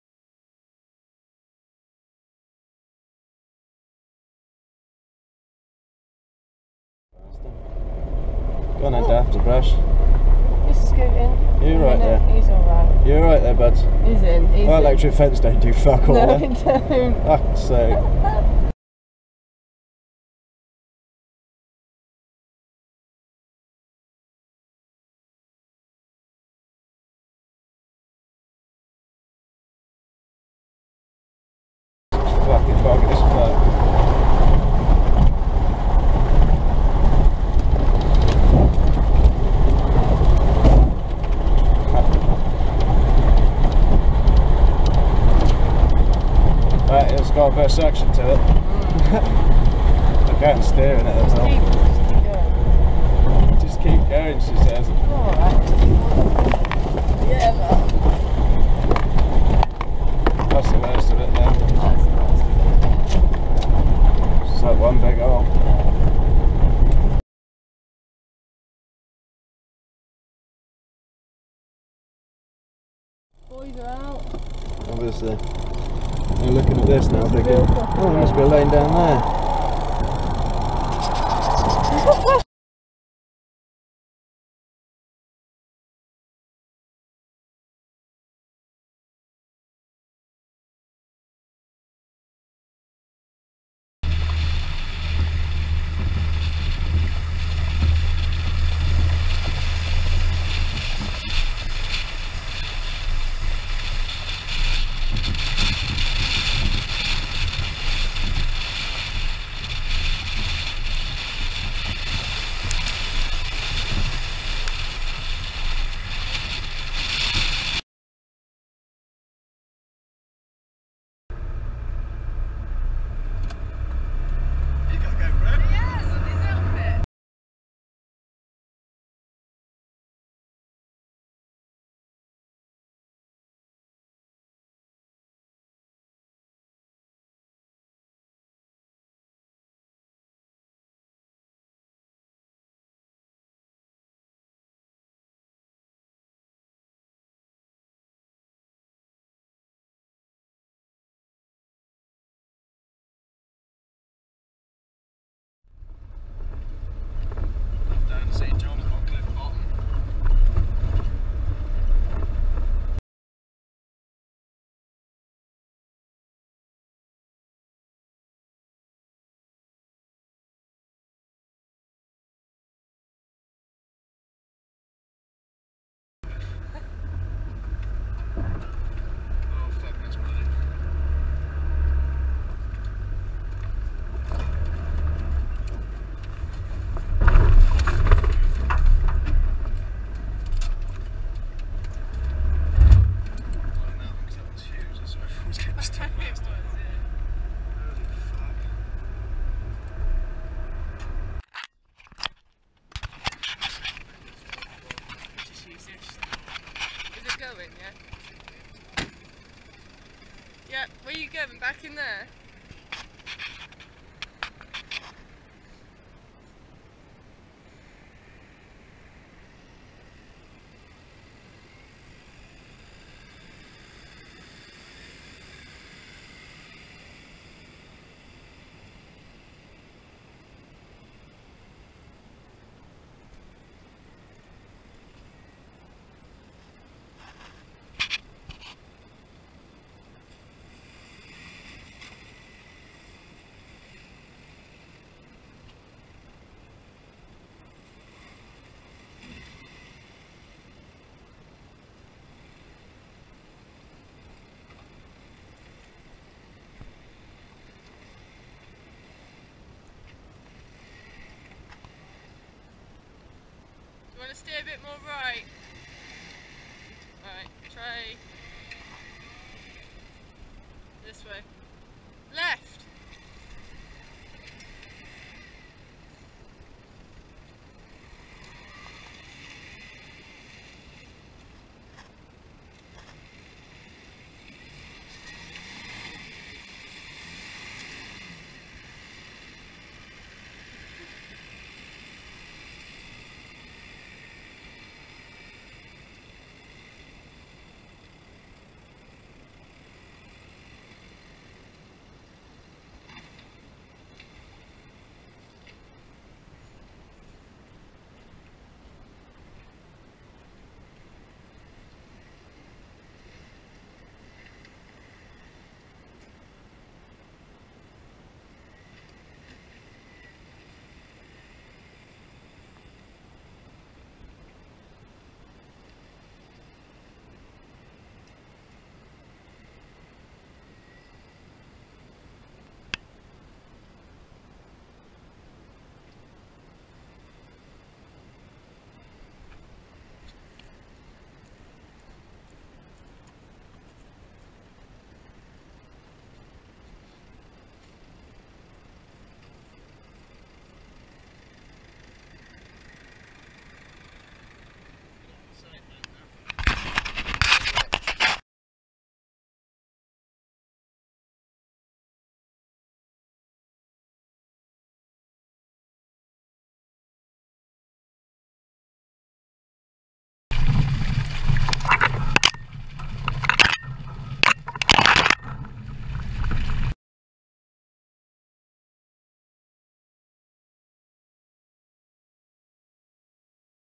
Gopro 4x4 Test in the Mud
All legal byways, last section filmed on a mates farm